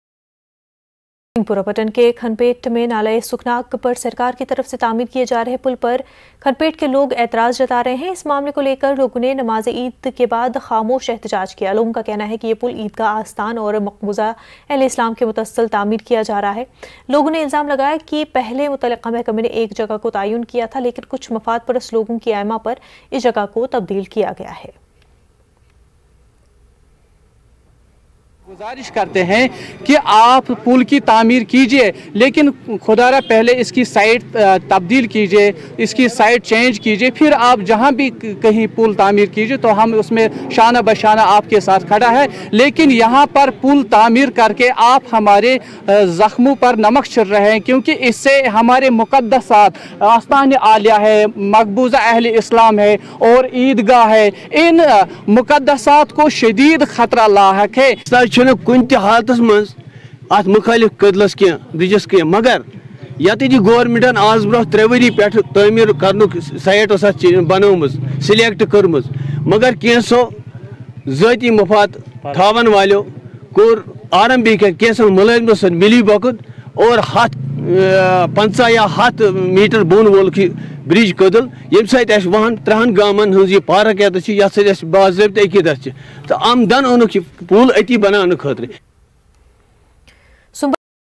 کھن پیٹھ میں نالہ سکھ ناگ پر سرکار کی طرف سے تعمیر کئے جارہے پل پر لوگ اعتراض جتا رہے ہیں
سنگھ پورہ پٹن کے کھن پیٹھ میں نالہ سکھ ناگ پر سرکار کی طرف سے تعمیر کئے جارہے پل پر کھن پیٹھ کے لوگ اعتراض جتا رہے ہیں۔ اس معاملے کولیکر آج یہاں لوگوں نے نماز عید کے بعد خاموش احتجاج کیا